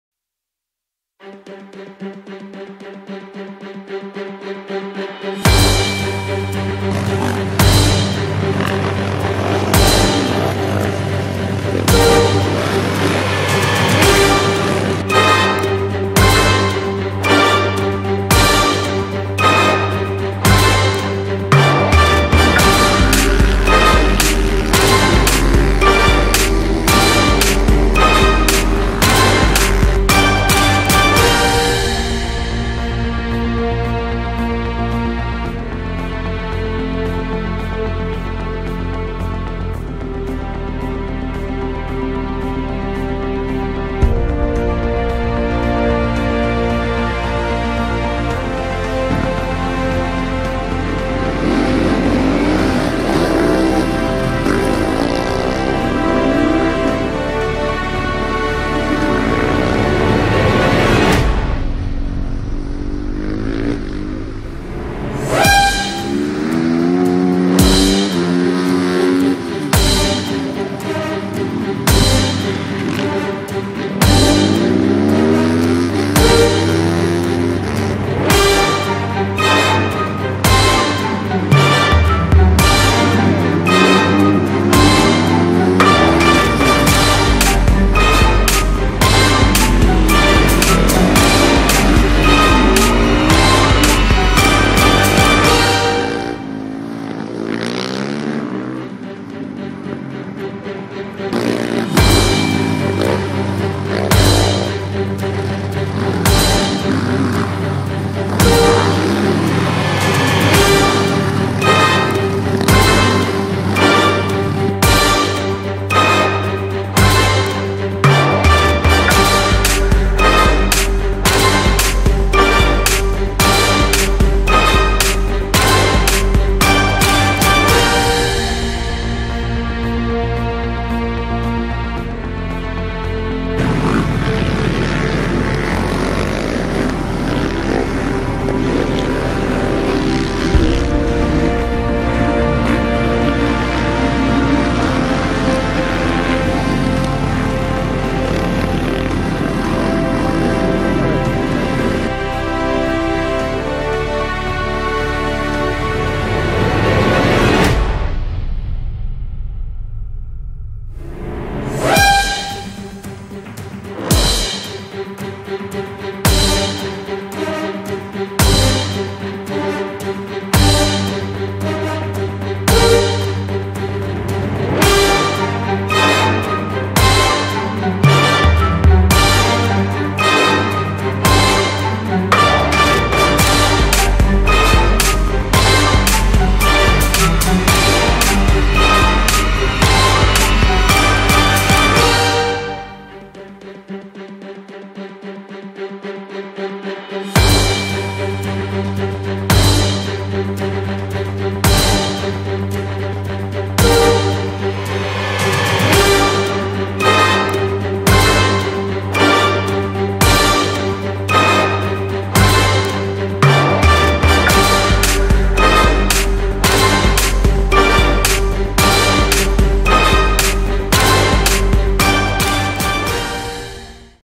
Moncalieri Warrior
Juju en course sur la piste Supermotard de Moncalieri (TO, Italie)